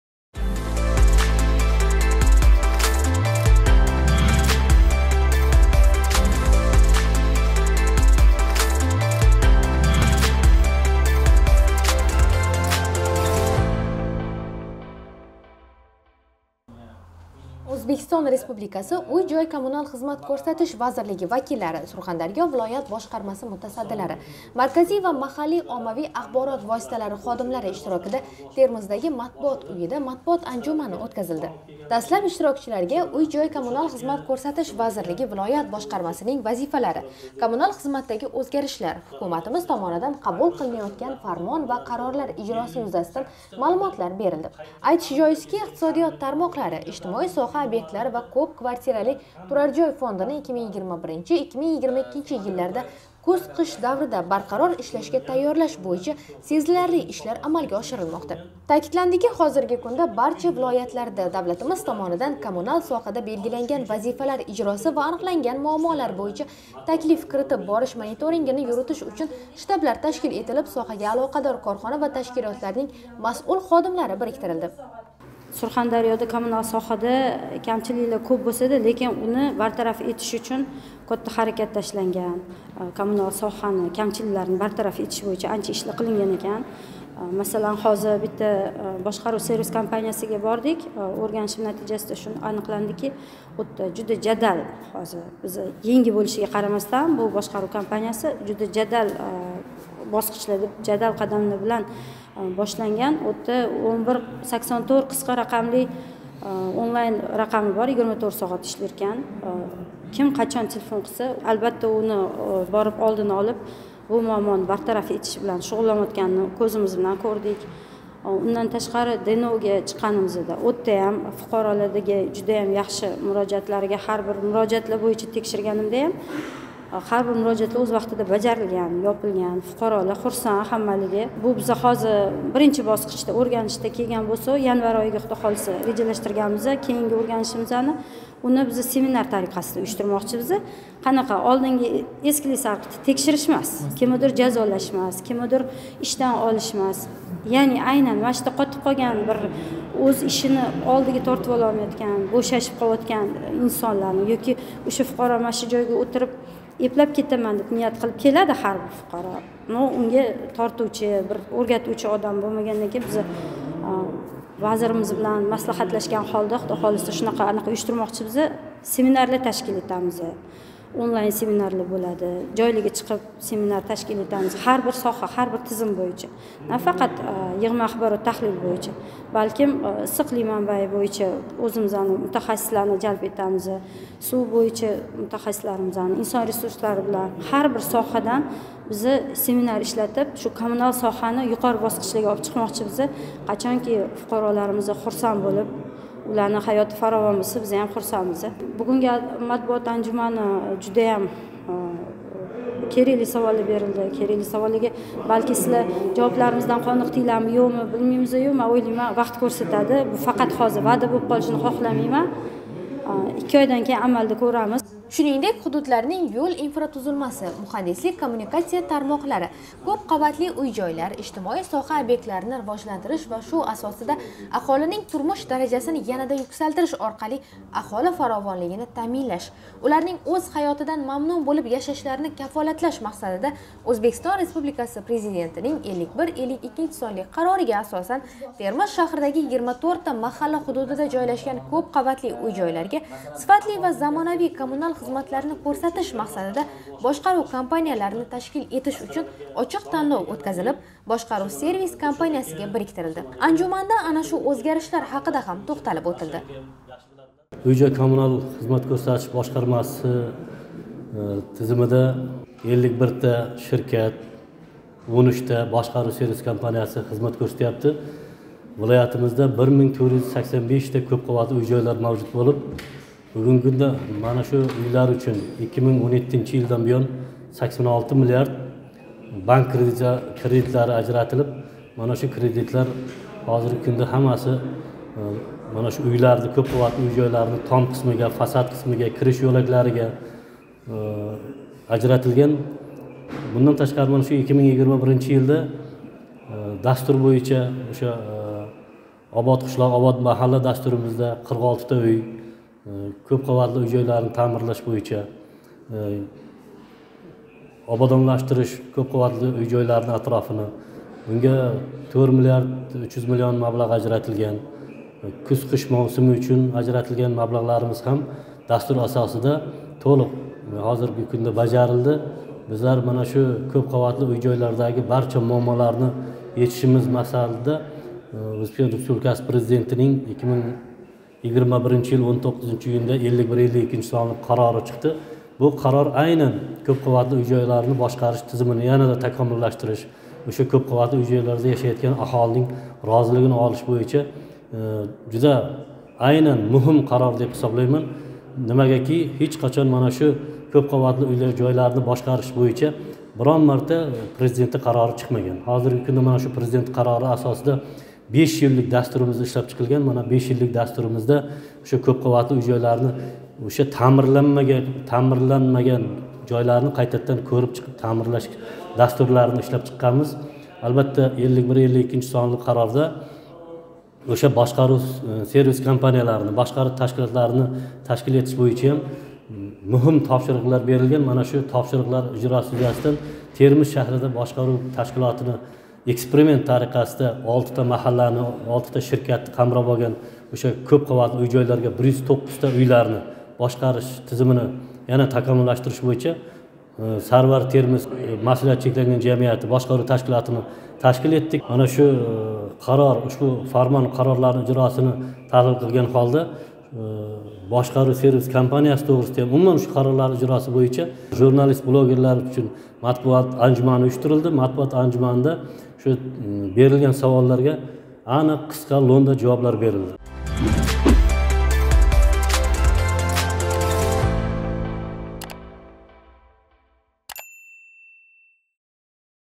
МАТБУОТ АНЖУМАНИ ЎТКАЗИЛДИ
Кеча Ўзбекистон Республикаси Уй-жой коммунал хизмат кўрсатиш вазирлиги вакиллари, Сурхондарё вилоят бошқармаси мутасаддилари, марказий ва маҳаллий оммавий ахборот воситалари ходимлари иштирокида Термиздаги Матбуот уйида матбуот анжумани ўтказилди.